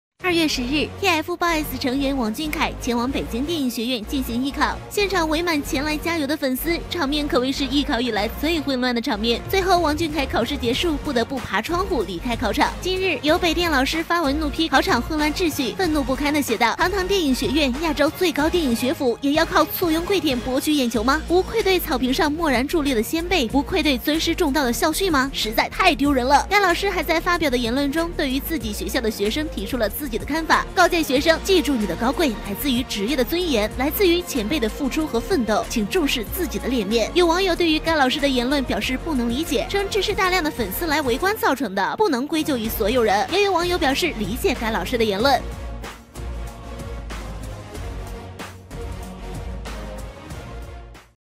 【3年前】北電老師斥藝考混亂：跪舔垃圾愧對先輩和校訓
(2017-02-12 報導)

2月10日，TFBOYS成員王俊凱前往北京電影學院進行藝考，現場圍滿前來加油的粉絲場面可謂是藝考以來最混亂的場面。最后王俊凱考試結束不得不爬窗戶離開考場。